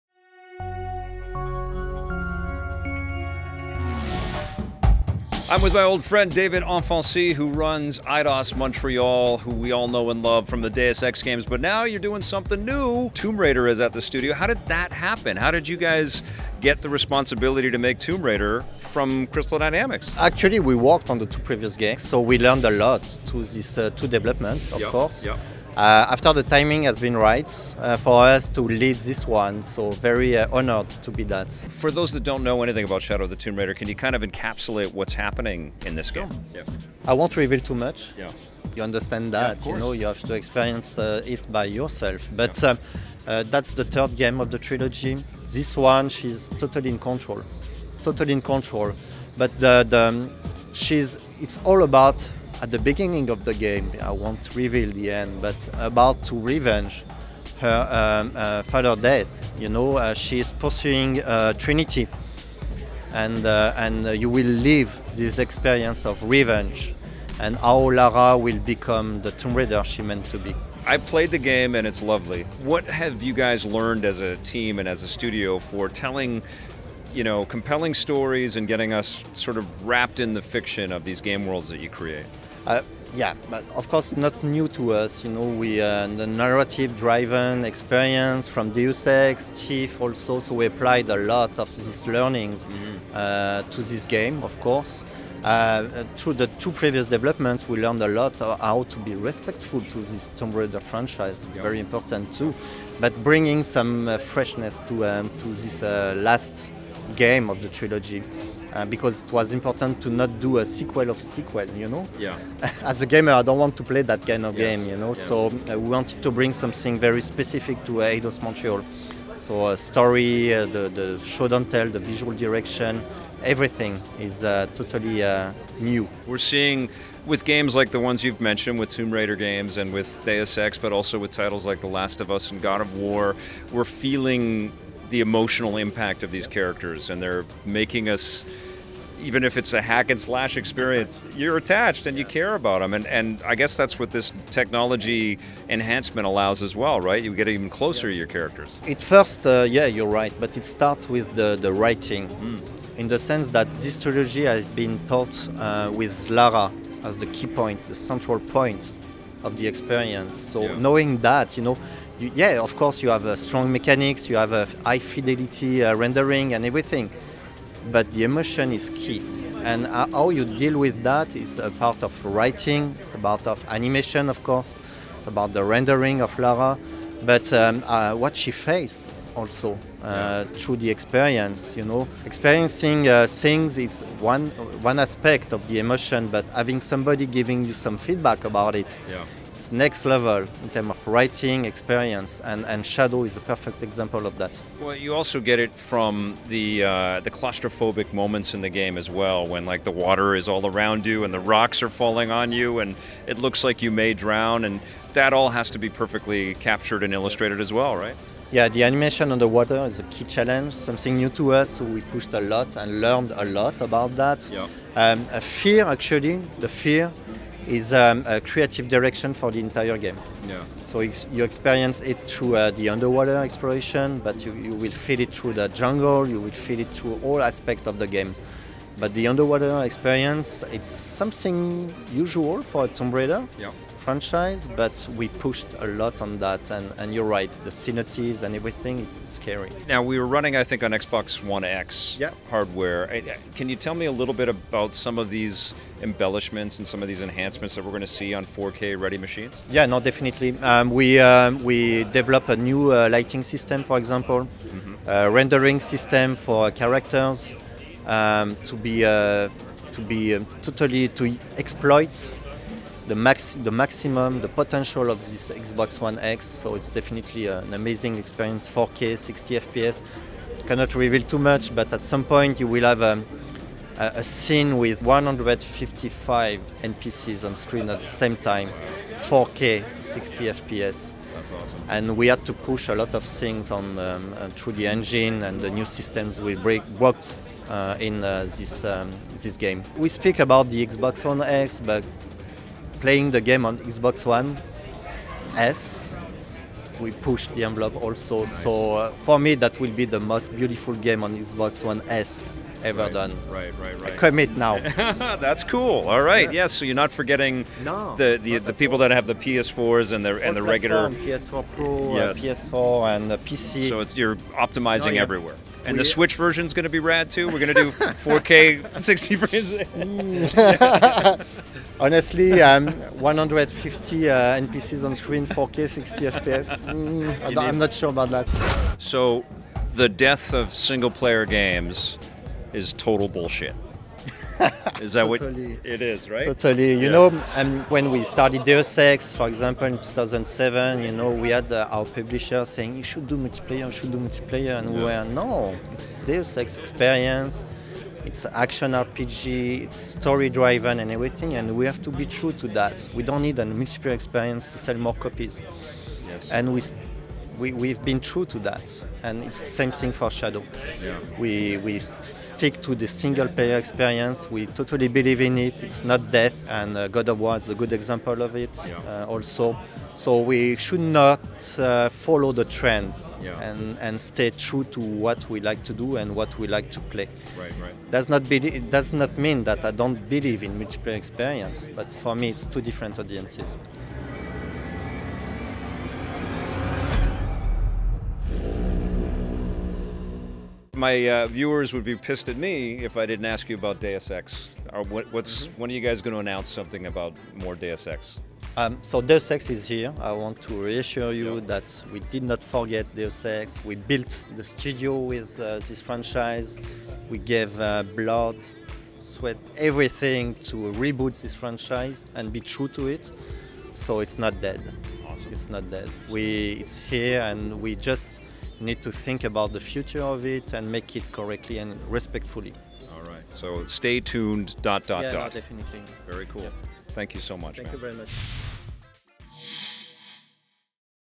What to Expect from Shadow of the Tomb Raider! - Electric Playground Interview
Victor Lucas speaks with Eidos Montreal studio head David Anfossi about Lara Croft's latest adventure, Shadow of the Tomb Raider!

We get several pieces of exclusive news about the game in this interview. Here are some highlights:

This will be the "last game in the trilogy" 1:42
It will run at 4K, 60fps on Xbox One X and PS4 Pro 4:26
The developers will "stick to the single player experience" 6:00
What about the future of Deus Ex? 6:31